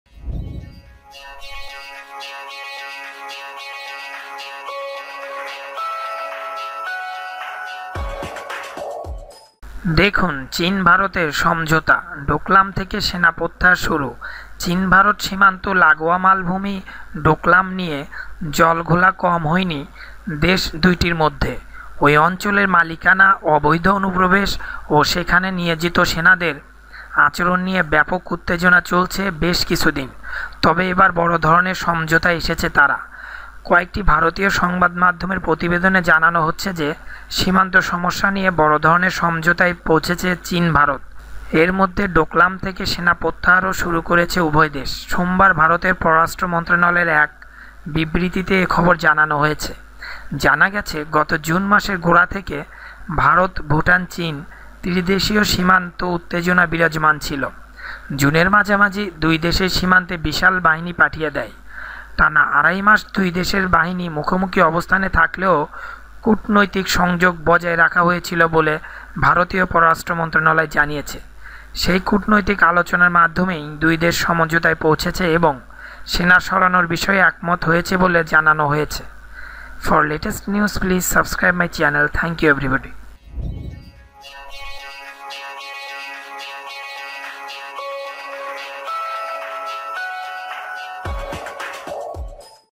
দেখুন চীন ভারতের সমঝোতা, ডোকালাম থেকে সেনা প্রত্যাহার শুরু
চীন-ভারত সীমান্ত লাগোয়া মালভূমি ডোকালাম নিয়ে জলঘোলা কম হয়নি দেশ দুটির মধ্যে। ওই অঞ্চলের মালিকানা, অবৈধ অনুপ্রবেশ ও সেখানে নিয়োজিত সেনাদের আচরণ নিয়ে ব্যাপক উত্তেজনা চলেছে বেশ কিছুদিন। তবে এবার বড় ধরণের সমঝোতায় এসেছে তারা।

কয়েকটি ভারতীয় সংবাদমাধ্যমের প্রতিবেদনে জানানো হচ্ছে যে, সীমান্ত সমস্যা নিয়ে বড় ধরণের সমঝোতায় পৌঁছেছে চীন-ভারত। এর মধ্যে, ডোকালাম থেকে সেনা প্রত্যাহারও শুরু করেছে উভয় দেশ। সোমবার ভারতের পররাষ্ট্র মন্ত্রণালয়ের এক বিবৃতিতে এ খবর জানানো হয়েছে।
জানা গেছে, গত জুন মাসের গোড়া থেকে ভারত-ভুটান-চীন ত্রিদেশীয় সীমান্তে উত্তেজনা বিরাজমান ছিল। জুনের মাঝামাঝি দু’দেশই সীমান্তে বিশাল বাহিনী পাঠিয়ে দেয়। টানা আড়াই মাস দু’দেশের বাহিনী মুখোমুখি অবস্থানে থাকলেও কূটনৈতিক সংযোগ বজায় রাখা হয়েছিল বলে ভারতীয় পররাষ্ট্র মন্ত্রণালয় জানিয়েছে। সেই কূটনৈতিক আলোচনার মাধ্যমেই দু’দেশ সমঝোতায় পৌঁছেছে এবং সেনা সরানোর বিষয়ে একমত হয়েছে বলে জানানো হয়েছে।